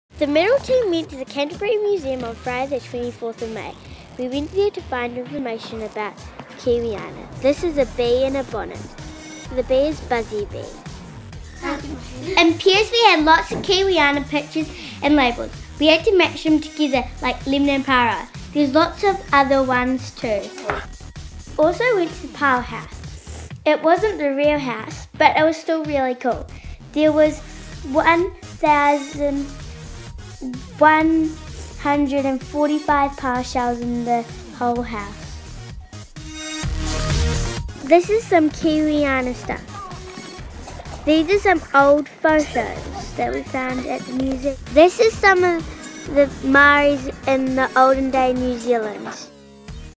Kiwiana Movie
This is what Maggie, Chloe and Jack discovered about Kiwiana at the Canterbury Museum.